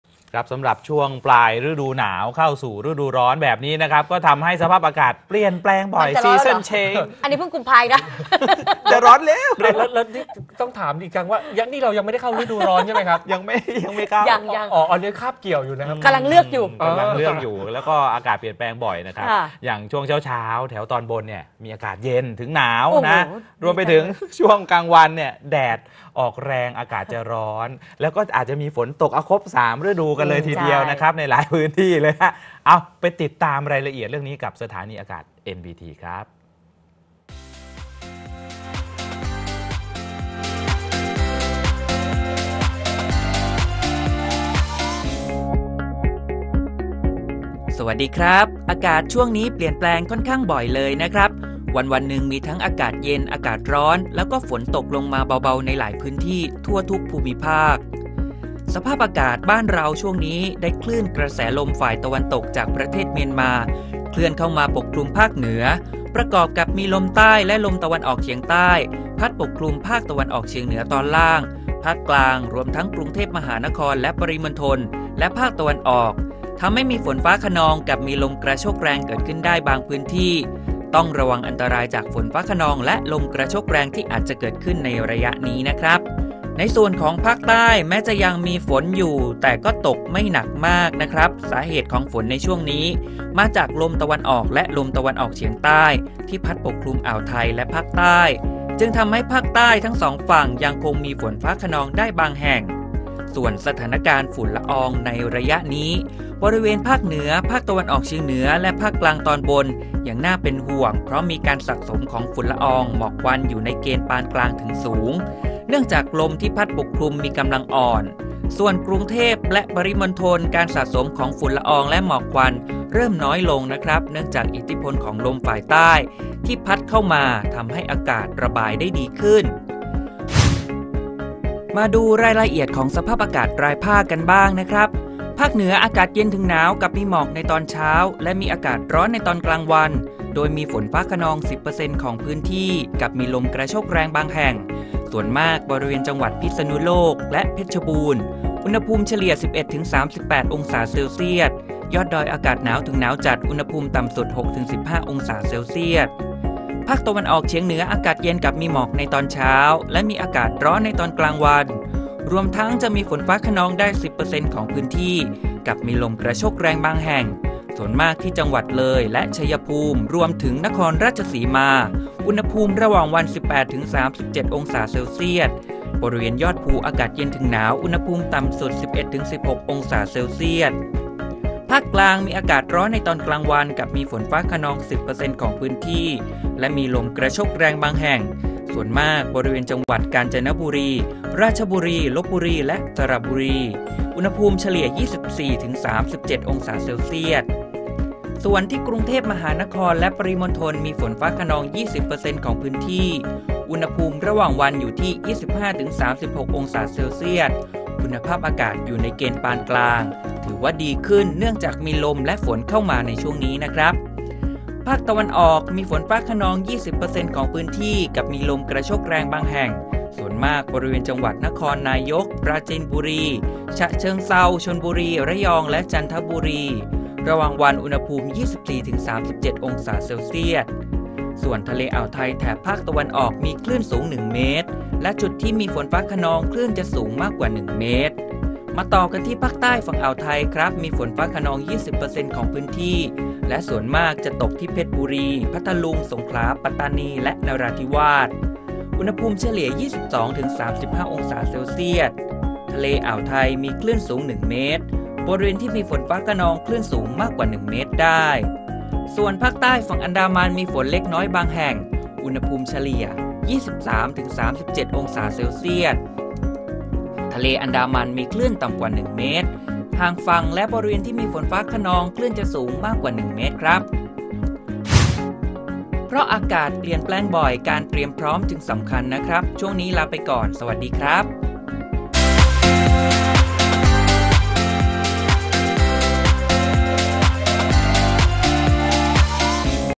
สถานีอากาศ NBT ข่าวเช้า วันที่ 19 กุมภาพันธ์ 2567
ช่วงปลายฤดูหนาวเข้าสู่ฤดูร้อนแบบนี้ทำให้อากาศเปลี่ยนแปลงบ่อย นะครับ/ค่ะ ช่วงเช้าๆแถวตอนบนจะมีอากาศเย็นถึงหนาว พอถึงช่วงกลางวันแดดแรงอากาศร้อน และมีฝนตกลงมาในหลายพื้นที่ด้วย ไปติดตามรายละเอียดกับสถานีอากาศ nbt